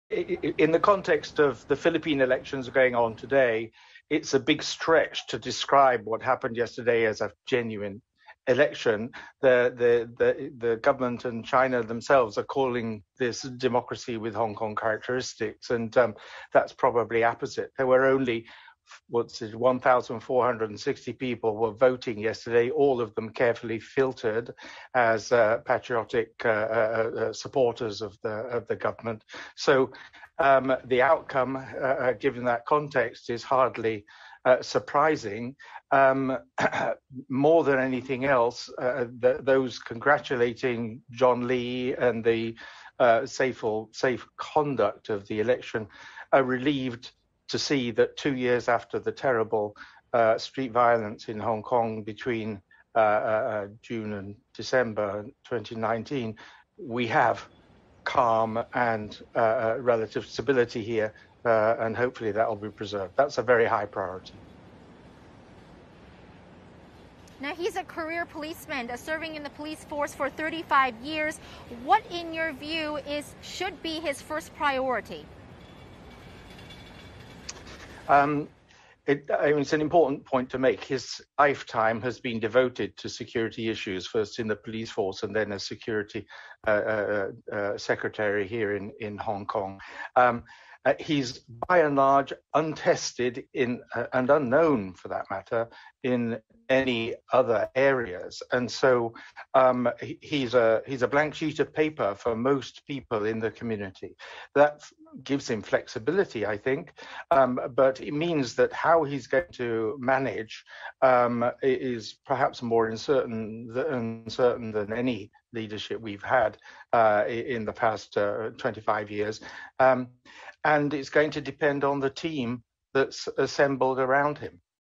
It would be a 'big stretch' to describe Hong Kong's election as genuine, says consultancy
David Dodwell of Strategic Access says it's apposite that the government in China itself is calling it a "democracy with Hong Kong characteristics."